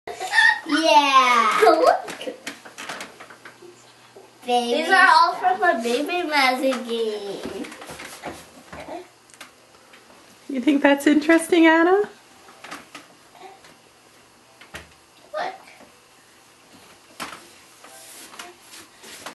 Sibling Baby Play Baby School
Baby school: a baby play idea that encourages siblings to be responsible nurturing.